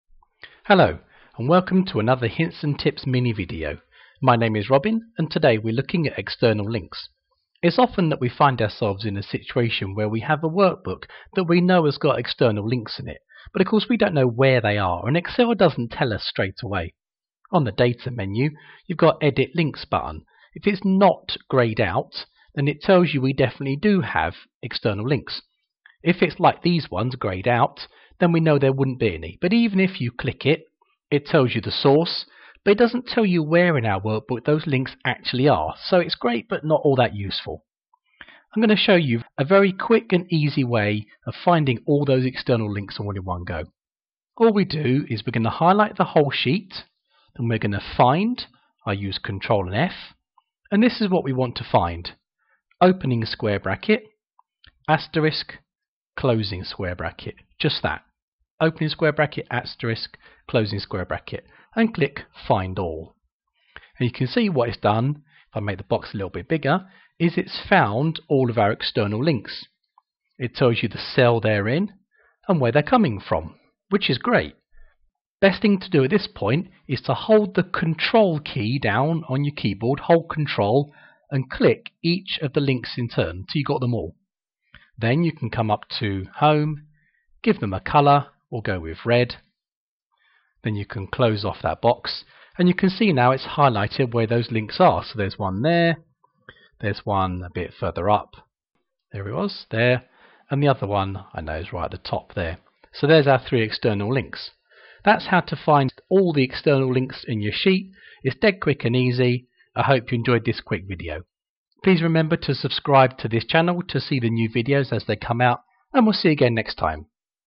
Excel Help - Finding all the external links in a worksheet
Excel doesn't automatically tell you where all the exetrnal links in a worksheet are. Use this trick to find them all in one go!